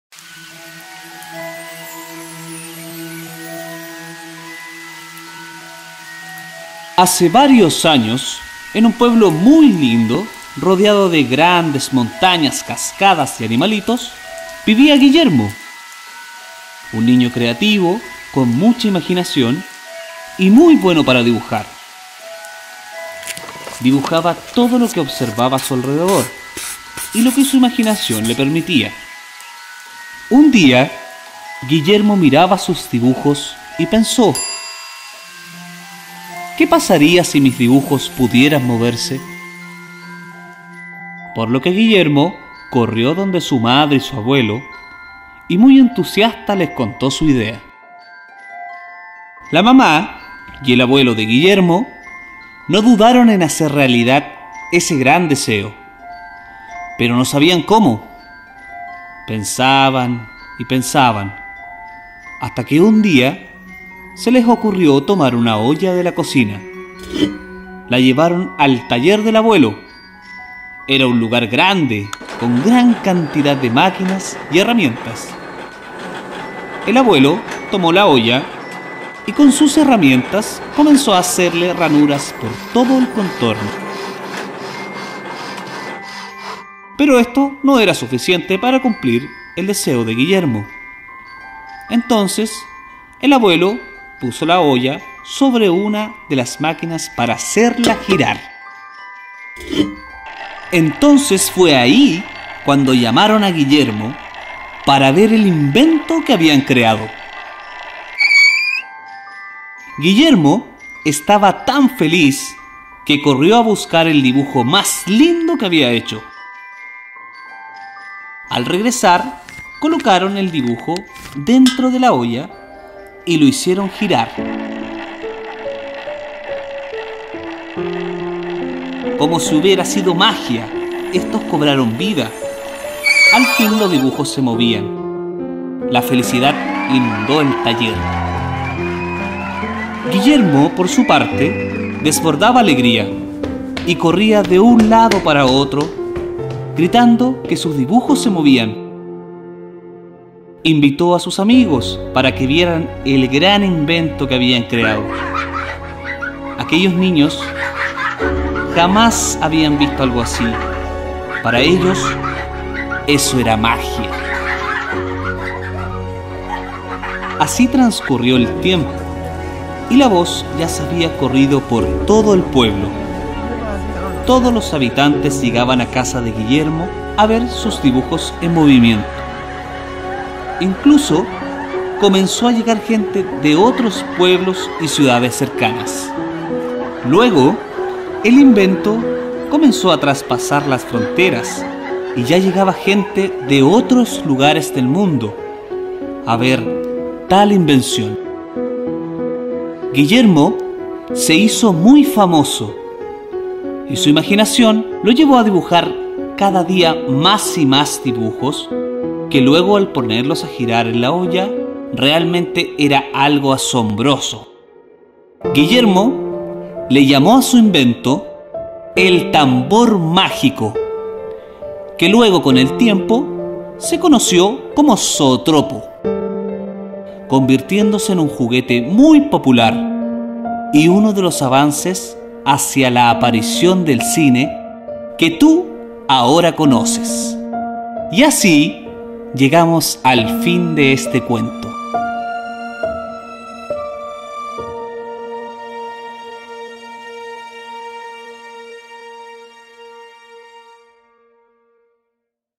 CuentaConCiencia: El mágico inicio del cine
¡¡Inauguramos los podcast en el MIM 🎧!!, con un cuento pensado para niños y niñas, que trata sobre el cine. Es ideal para antes de dormir, por ejemplo; solo le pones play y escuchan en familia. Esta nueva sección se llama CuentaConCiencia y esta historia es "El mágico inicio del cine".